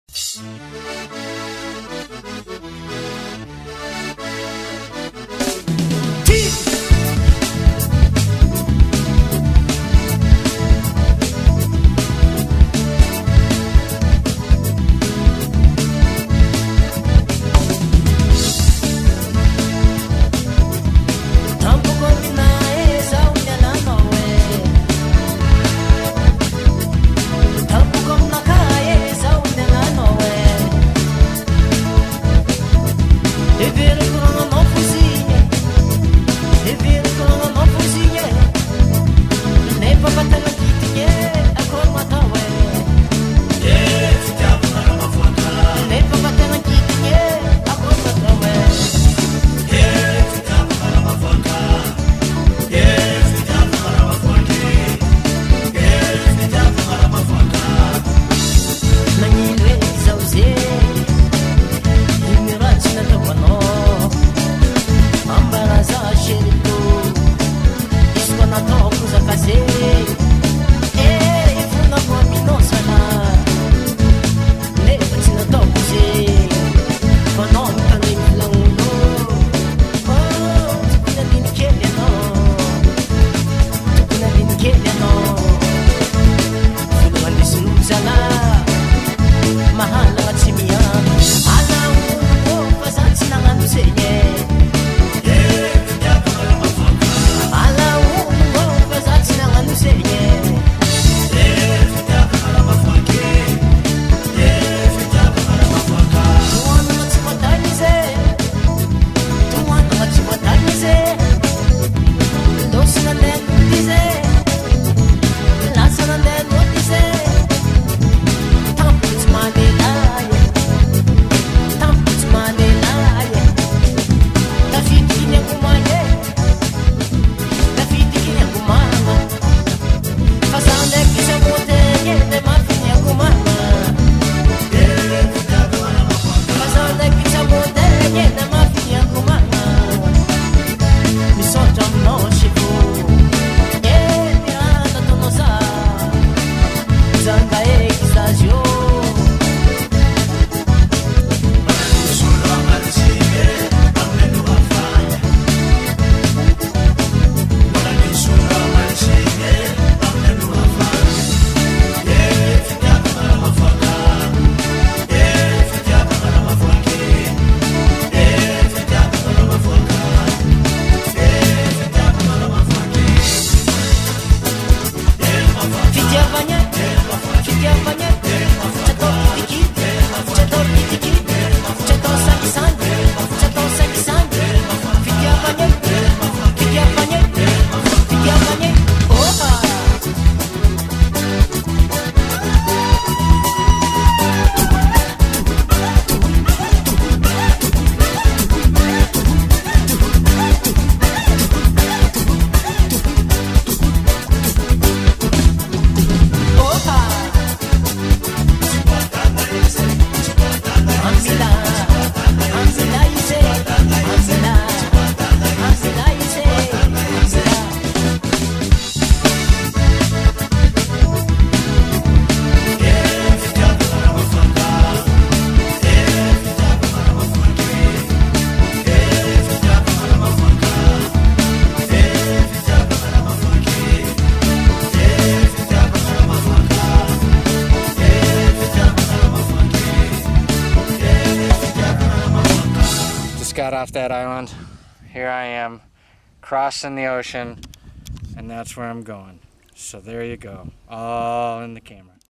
Madagascar Kayak V
In search of the "Lost Turtle" I spent 3 weeks kayaking in Madagascar. It was a journey in finding
myself after my divorce and the secrets that would heal me.